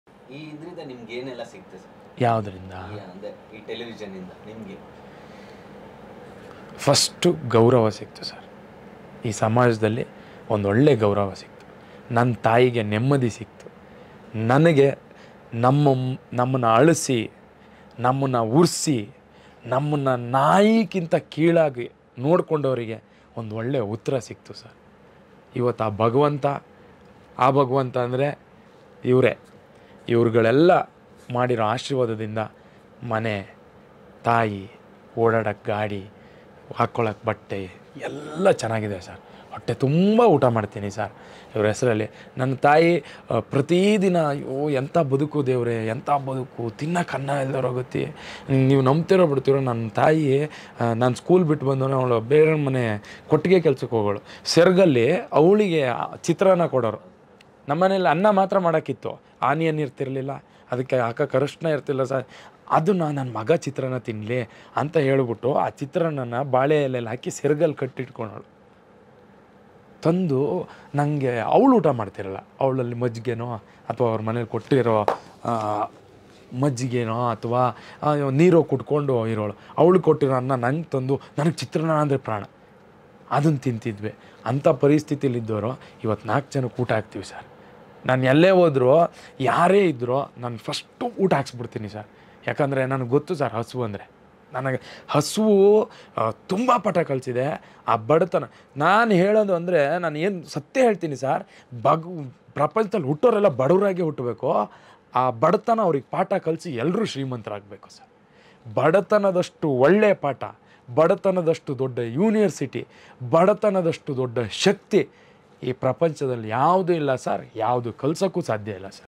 ನಾಯಿಗಿಂತ ಕಡೇದಾಗಿ ನೋಡಿದವರಿಗೆ...ಇವತ್ತು ಮನೆ ಕಾರು ಎಲ್ಲಾ ಕೊಟ್ಟಿದೆ ಹುಲಿ ಕಾರ್ತಿಕ ಭಾವುಕ ಮಾತುಗಳು | Karthik
ನಾಯಿಗಿಂತ ಕಡೇದಾಗಿ ನೋಡಿದವರಿಗೆ...ಇವತ್ತು ಮನೆ ಕಾರು ಎಲ್ಲಾ ಕೊಟ್ಟಿದೆ ಹುಲಿ ಕಾರ್ತಿಕ ಭಾವುಕ ಮಾತುಗಳು | Huli Karthik